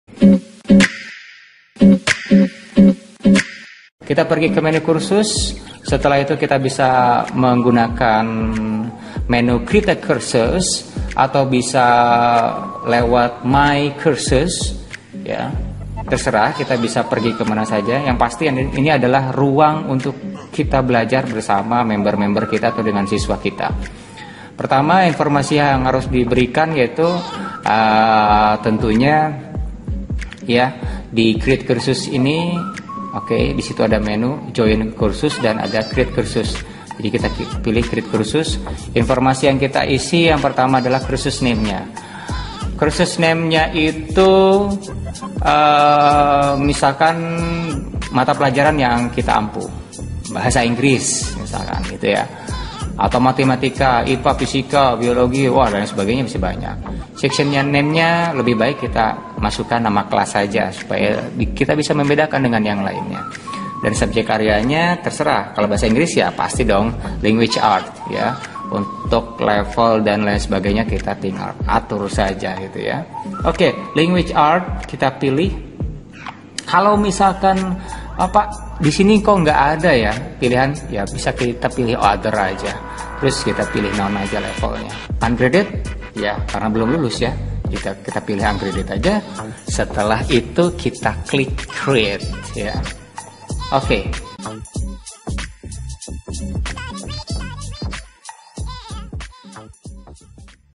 Membuat Ruang belajar di menu courses | Bahasa Tuntas Schoology Part 4
Pengenalan Schoology Untuk Dosen, Guru, & Pengajar.